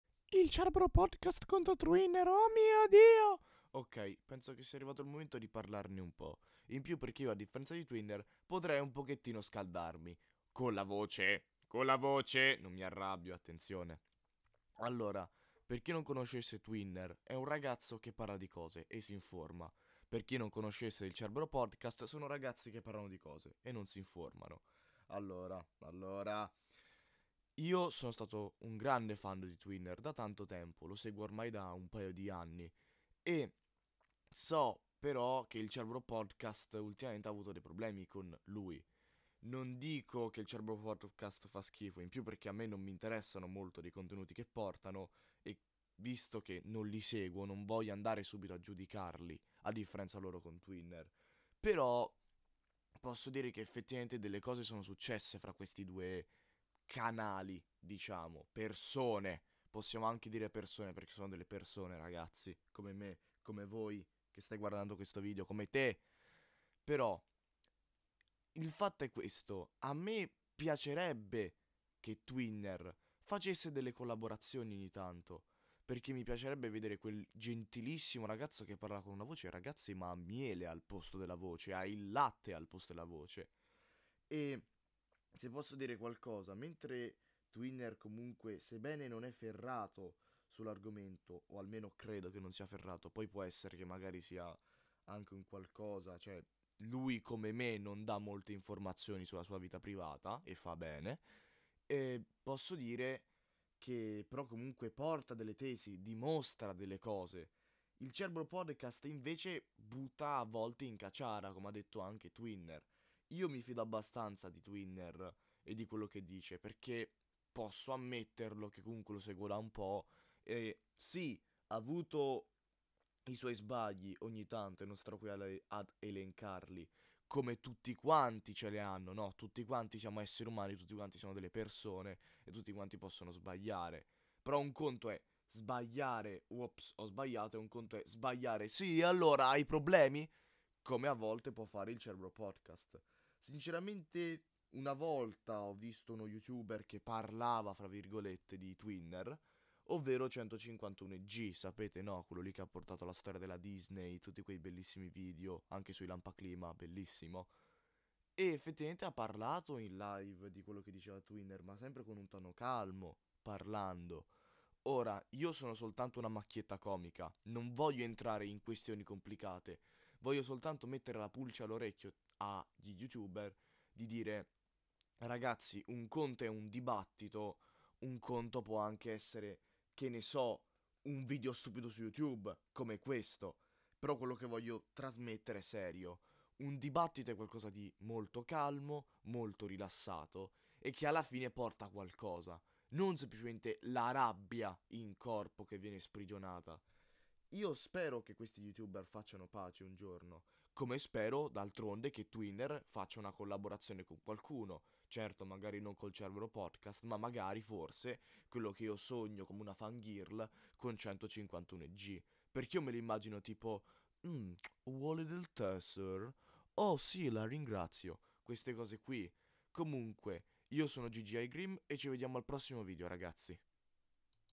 parliamo di TWEENER contro il CERBERO PODCAST ( la mia opinione- svarioni)
il video e senza scopo di offendere, in più perché se no mi buttano giù anche la casa, se vi offendete scusate.
p.s. la sedia a 2 gambe non è fatta di legno di pino, ma ben si di legno di quercia.